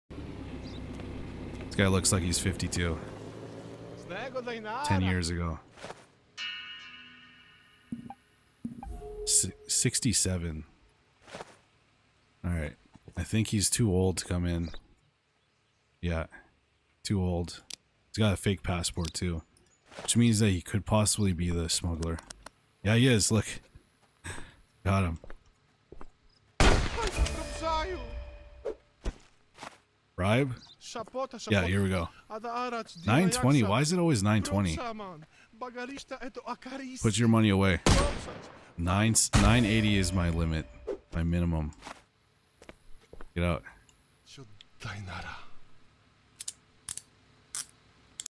980 minimum
The year is 1981, and as a young officer trainee, you are assigned to work at a border post in the mountainous region of Karikatka.

Entry into the Acarist People's Republic is strictly regulated by the party and your every move is monitored!

The situation at the border escalates when your comrade is killed during one of the interventions...

Document control:
Every visitor must have a set of valid documents. Even the slightest discrepancy means refusal of entry.

Over time, the number of documents will increase and errors will be more difficult to detect.

Smuggling detection:
Our intelligence regularly provides information about suspicious drivers. Search their vehicles using a UV flashlight and then use the appropriate tools to extract the smuggled goods.
No amount of contraband should cross the border!

Shootouts:
In some situations you will be forced to use firearms. Your station is a constant target for Oberankov's gang attacks, and every away mission carries the risk of a clash with the enemy.

Police chases:
Desperate smugglers may attempt to escape from the control zone. It is your duty to stop and arrest them!
Buy and upgrade police cars to improve the effectiveness of your pursuit.

Post management:
Upgrade your police station to increase its defenses and improve subsequent inspections. However, keep in mind the rising cost of maintaining your station.
Successful inspections, foiled smuggling and successful interventions are a few ways to earn cash.

Special assignments:
Always be ready to intervene in an emergency. Solve crime puzzles, support your comrades and thwart the Blood Fist rebels.
Make decisions that will determine the future of Acaristan.